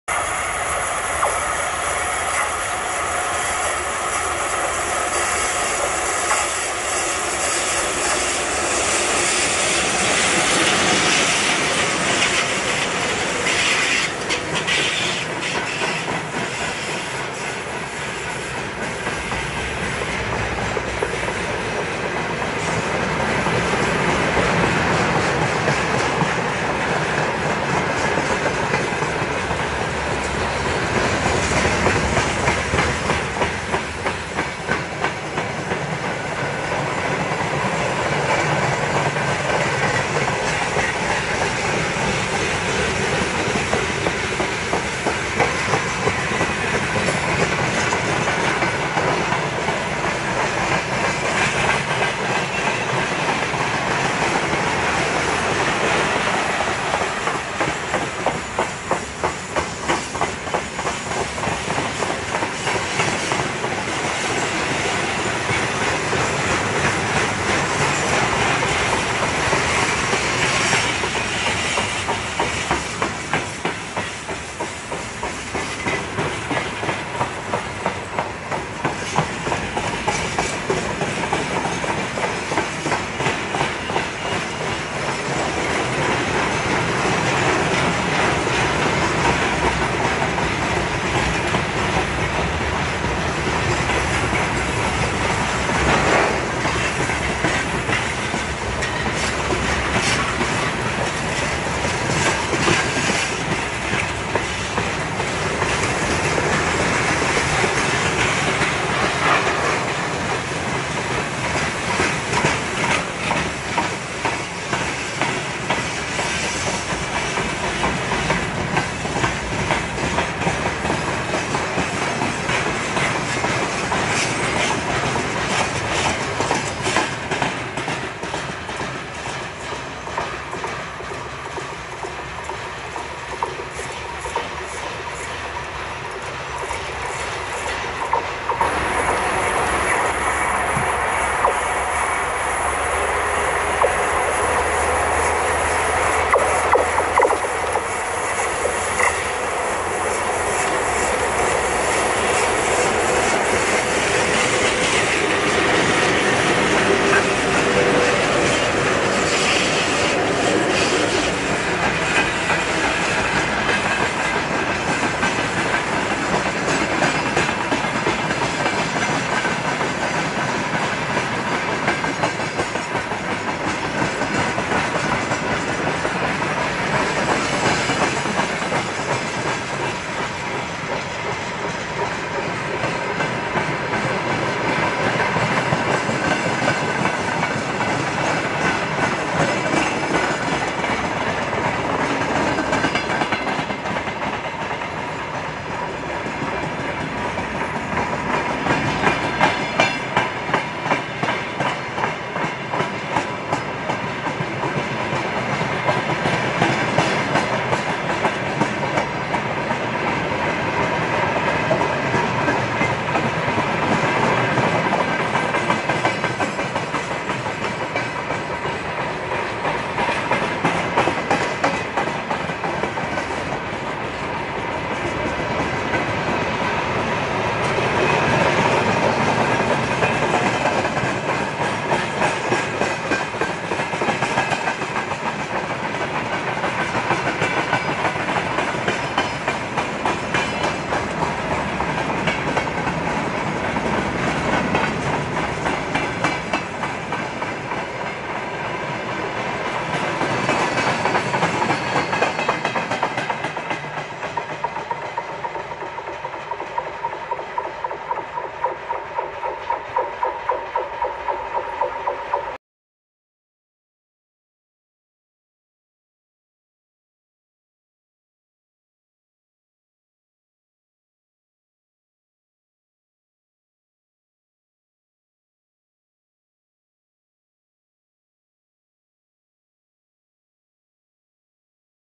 2024 July 7 Uzbekistan VL80 Freight train at near Tashkent ウズベキスタン鉄道トロピカルVL80 3重連タシケント近郊
2024 July   7

Uzbekistan VL80  Freight train
was taken  2video  at near  Tashkent

ウズベキスタン鉄道トロピカルVL80　3重連

タシケント近郊で撮影しました